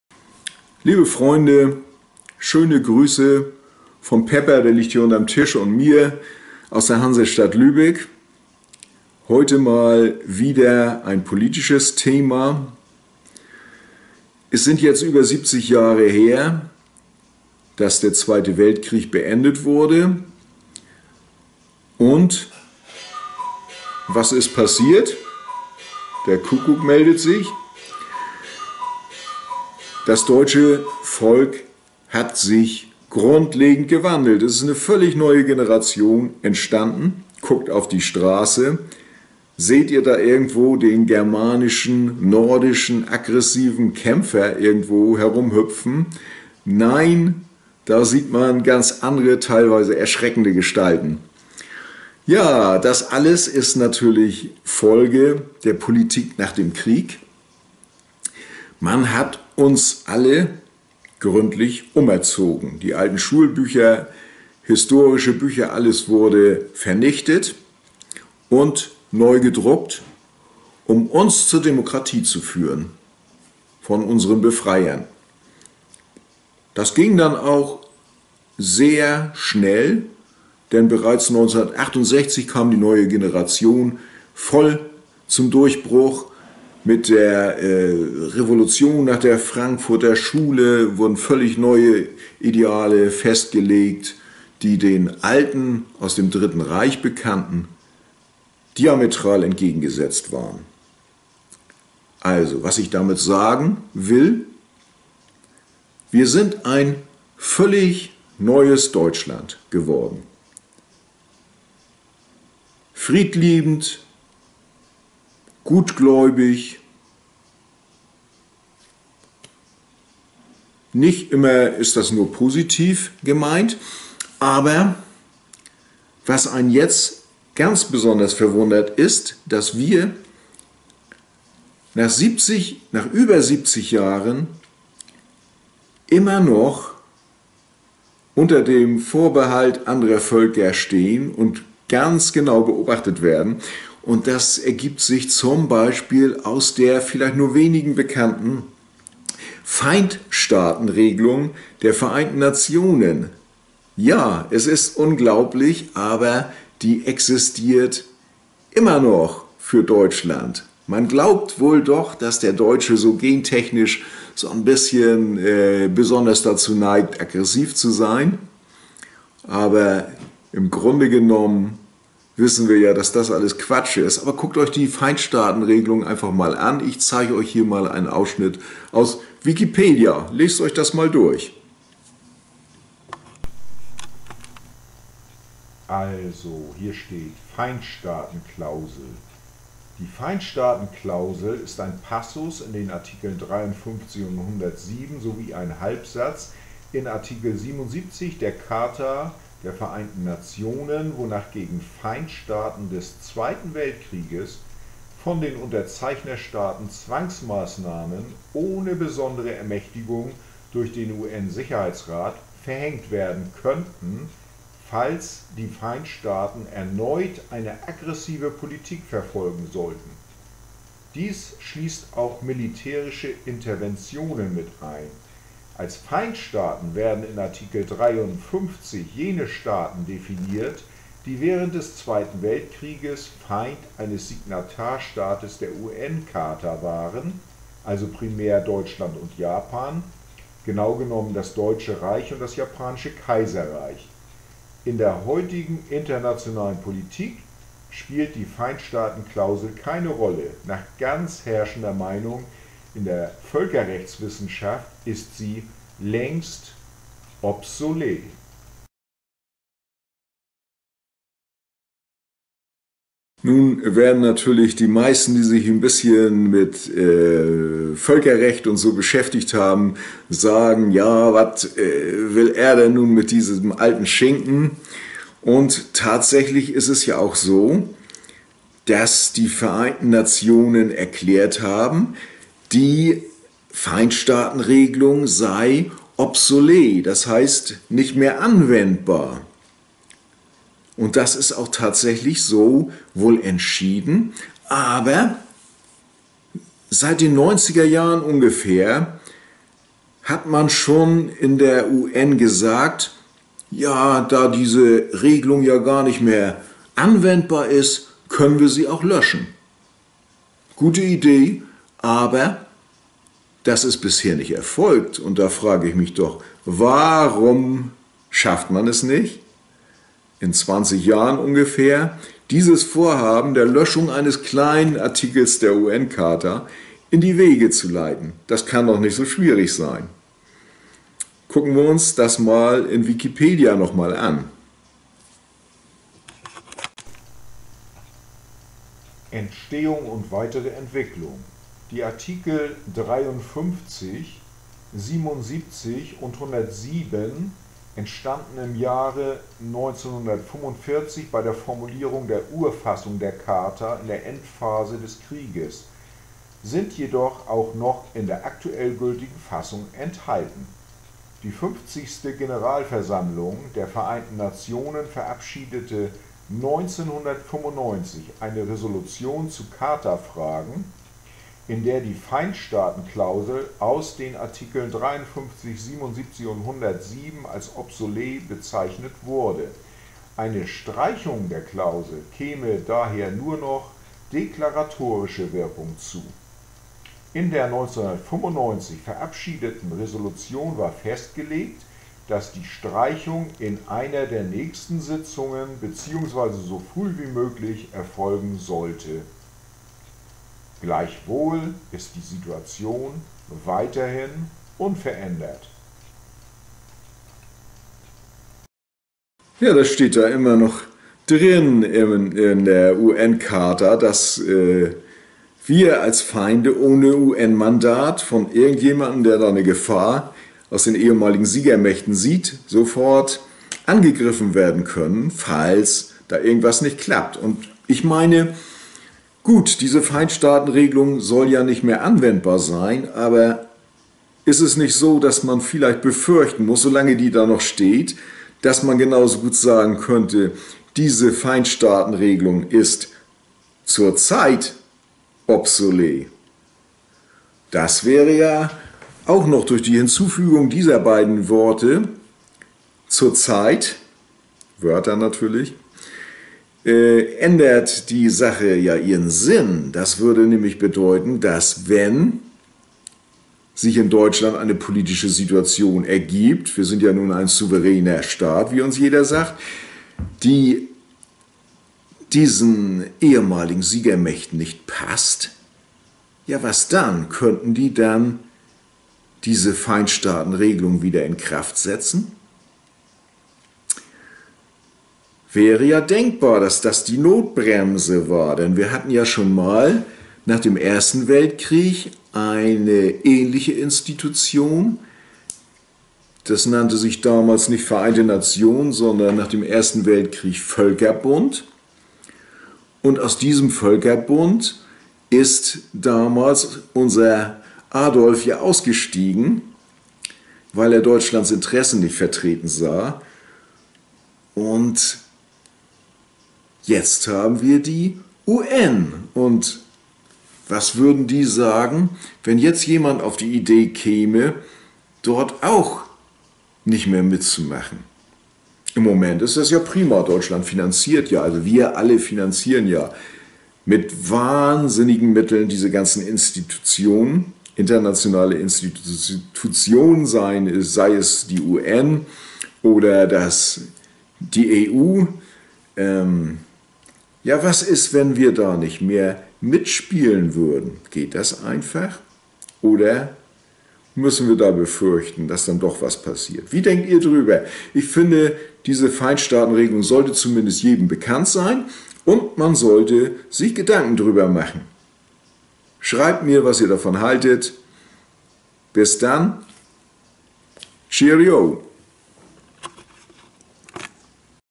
Die Feindstaatenregelung der Vereinten Nationen
1995 wollten die Vereinten Nationen die "Feindstaatenklausel", ein Relikt der Nachkriegszeit, streichen.
Das wäre sicherlich ein Beitrag zur Vergangenheitsbewältigung gewesen...

Sie gilt zwar als (zur Zeit ?) "obsolet", existiert aber heute noch - über 70 Jahre nach dem Ende der Weltkriege.

Was sollen wir davon halten?